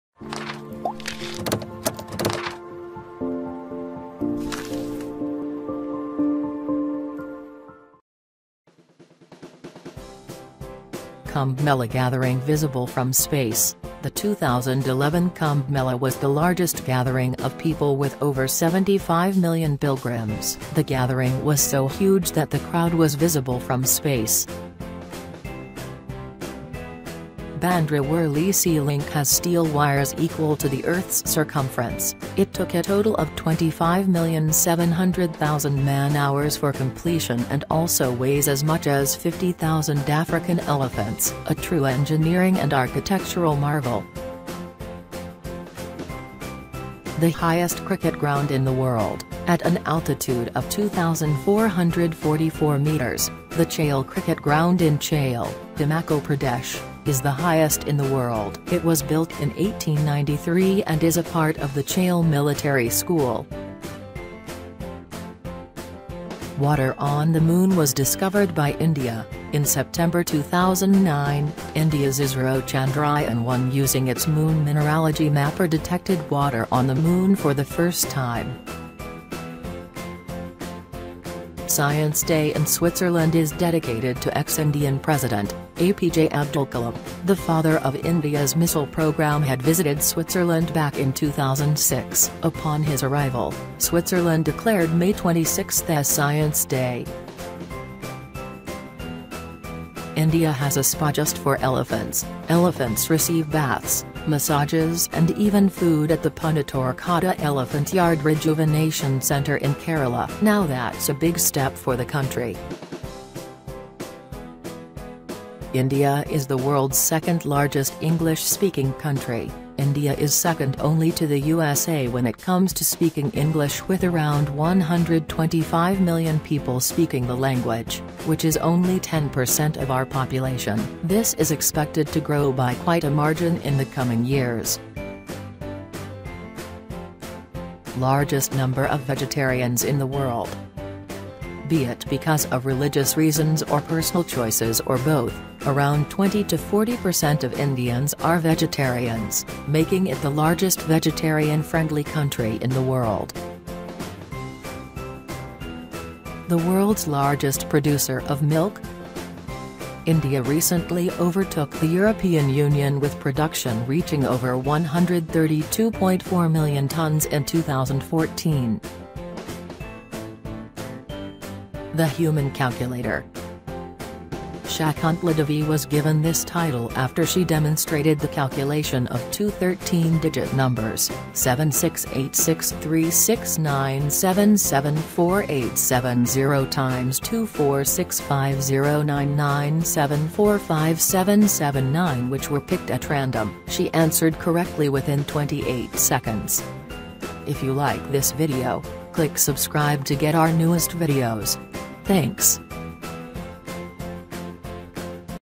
Top 10 Amazing Facts About India
According to UN estimates, India will become the most populous country in the world in just 14 years' time, when it will have about 1.45 billion inhabitants.
For many in India, becoming the most populous country will be an achievement, marking the country's progress in its rivalry with China.
For others, particularly from the older generations, it represents a failure of the country's decades-old attempts to bring its population under control - which included a controversial and counter-productive mass sterilisation campaign during the 1970s.
In fact, birth rates have fallen significantly in almost all parts of India, driven by female education, rising household incomes and greater availability of contraception though this has been partially offset by increased life expectancy.